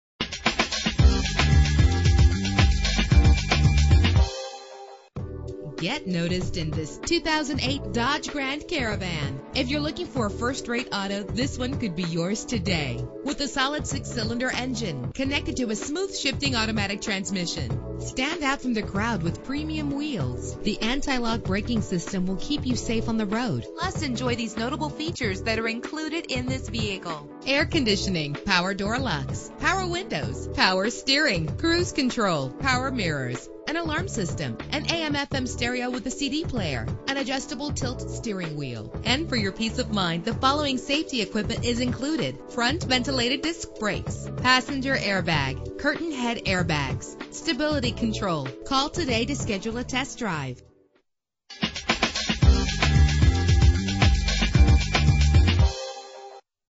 2008 Dodge Grand Caravan - Killeen TX
Year: 2008
Make: Dodge
Model: Grand Caravan
Trim: SXT
Engine: 3.8 liter 6 cylinder 12 valve
Transmission: Automatic
Color: Beige
Mileage: 63891
Address:
4001 E. Cen Tex Expwy